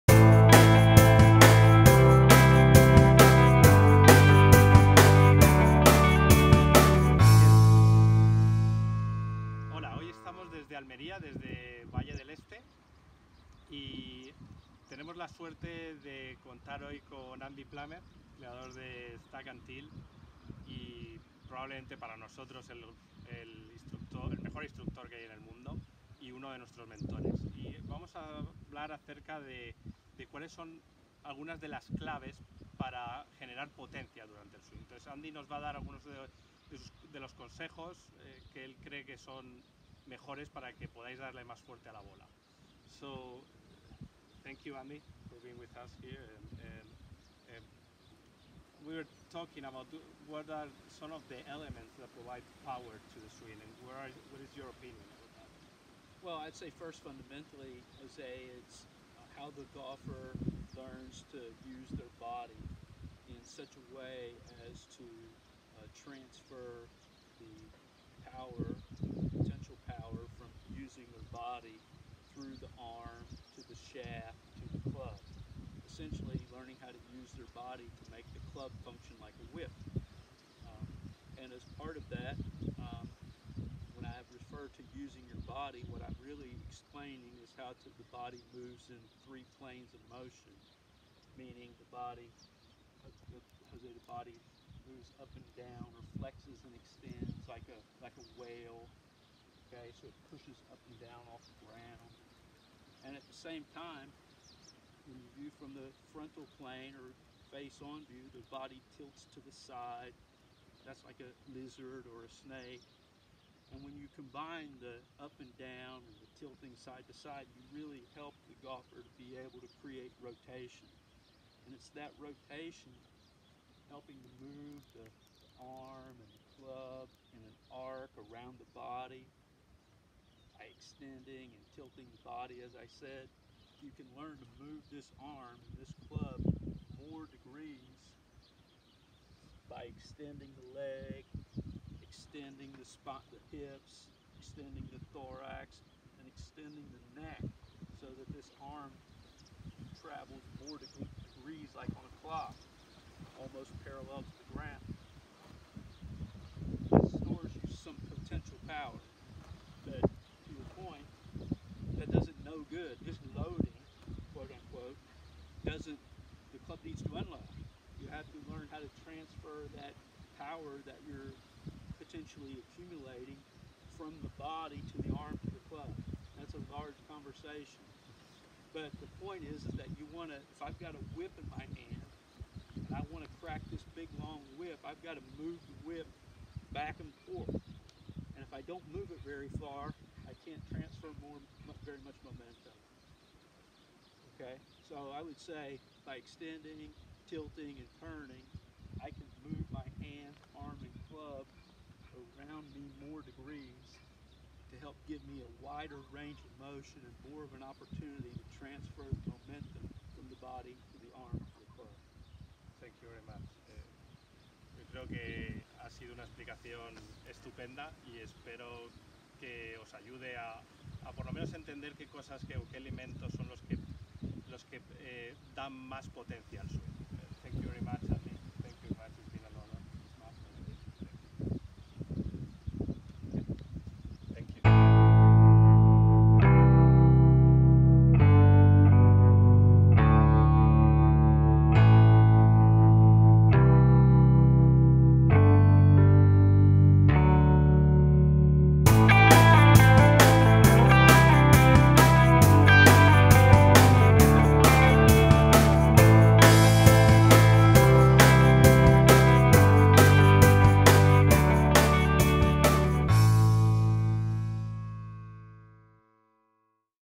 Como se crea potencia en el swing con Andy Plummer(Activar subtítulos)-Consejos de Golf en Español
Este vídeo trata de: Como se crea potencia en el swing con Andy Plummer (Activar subtítulos)